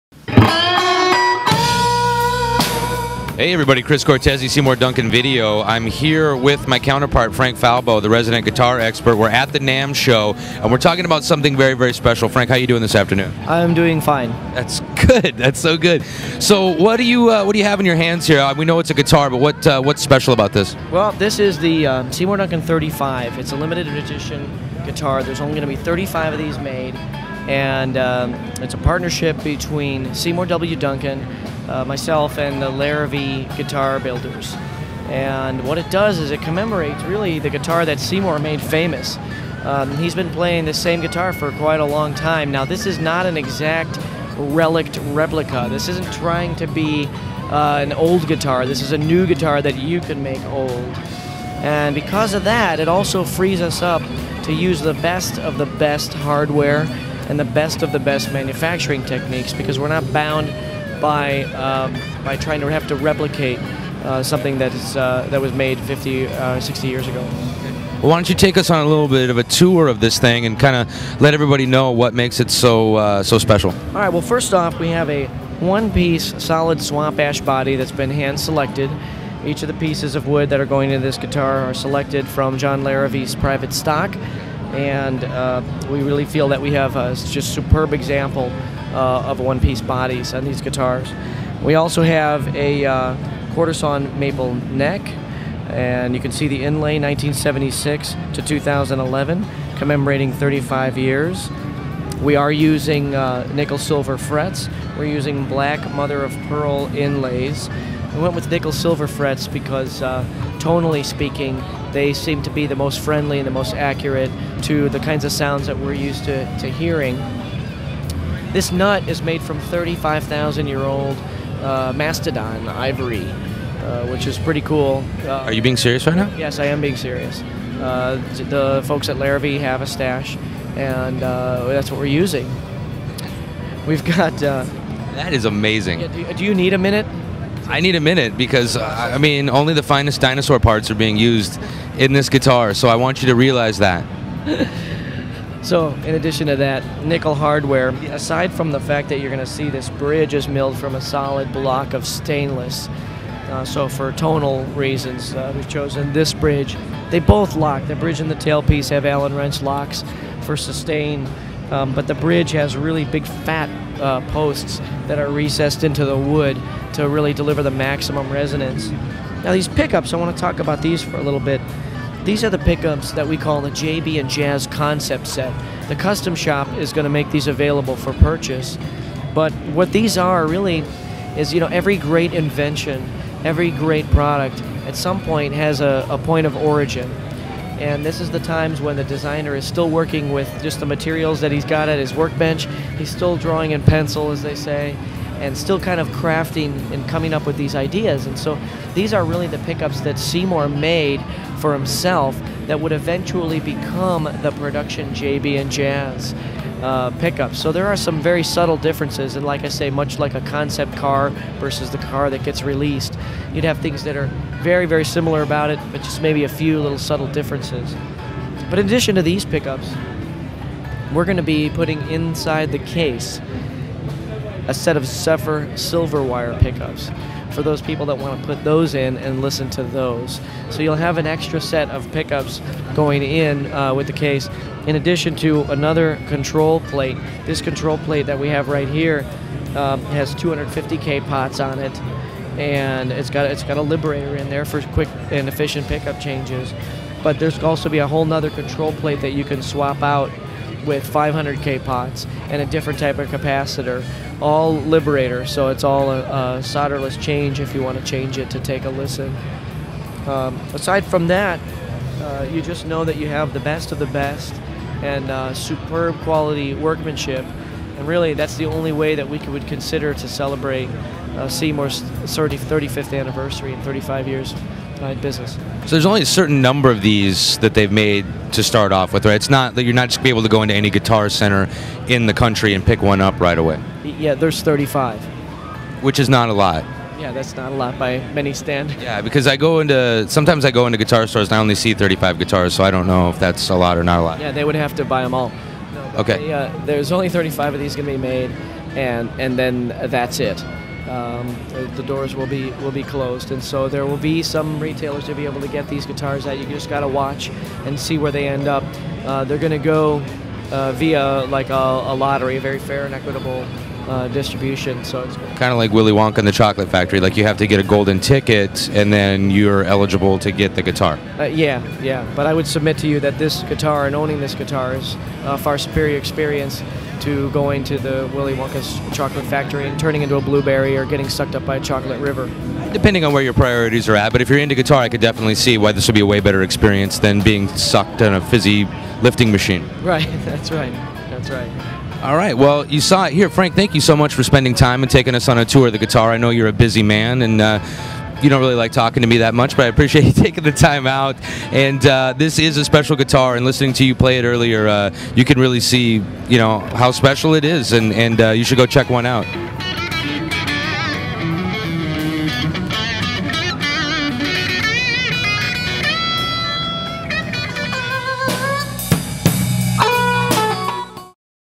Limited Edition Seymour Duncan 35 Guitar
Seymour Duncan's Frank Falbo talks about the new Seymour Duncan 35, a master crafted instrument strictly limited to 35 numbered, signed copies.